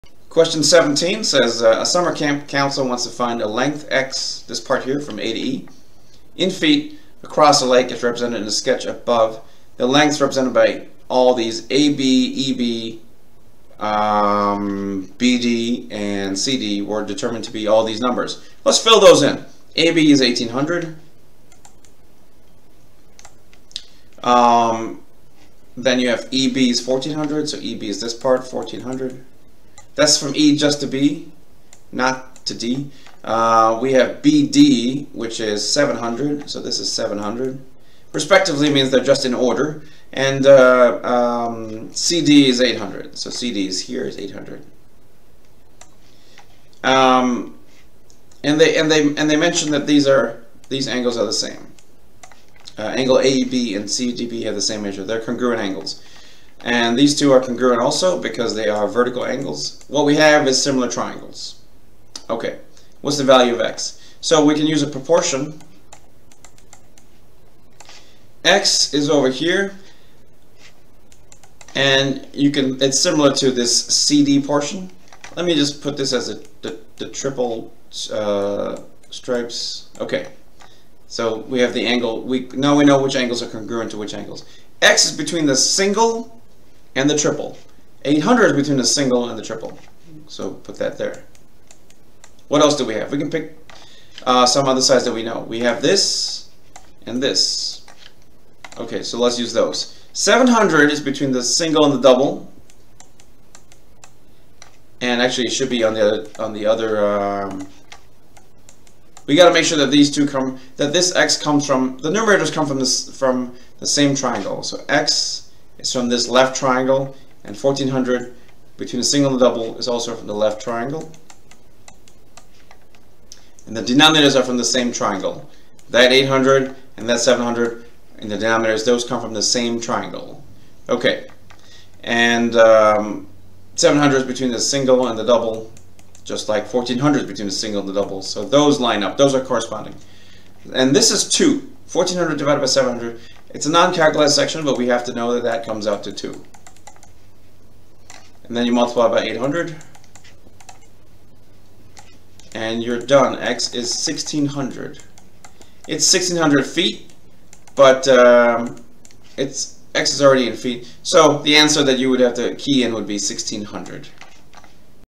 A summer camp counselor wants to find a length, x Q17 of SAT Math Practice Test 1 Without Calculator
A summer camp counselor wants to find a length, x, in feet, across a lake as represented in the sketch above. The lengths represented by AB, EB, BD, and CD on the sketch were determined to be 1800 feet, 1400 feet, 700 feet, and 800 feet, respectively. Segments AC and DE intersect at B, and ∠AEB and ∠CDB have the same measure. What is the value of x ?

Q17 of SAT Math Practice Test 1 Without Calculator